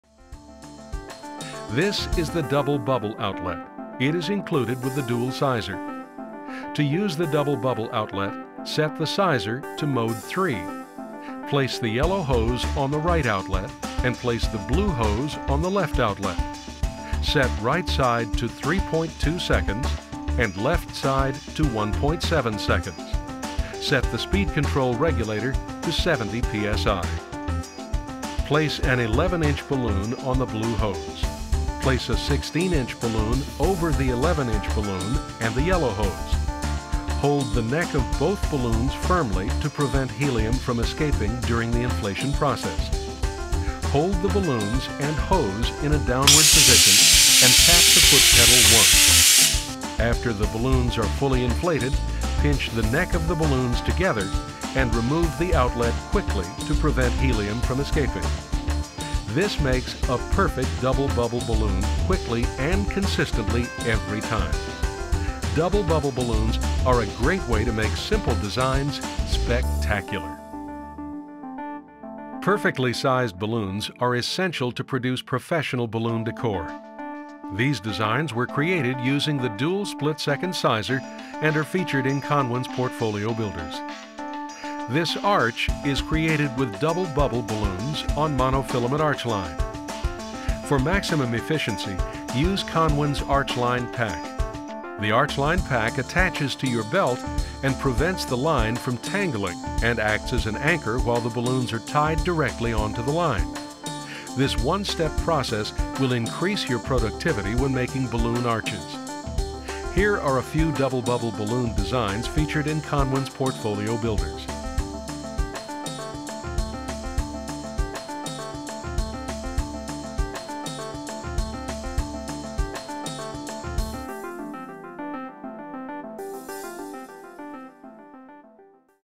Double Bubble Outlet for Conwin's Dual Split Second Sizer with Sue Bowler
The Double Bubble Outlet attaches to conwin's Dual Sizer and makes it easy to inflate 11" latex balloons inside 16" latex balloons at the tap of a foot pedal. This product is included with the original purchase of a Dual Split-Second Sizer.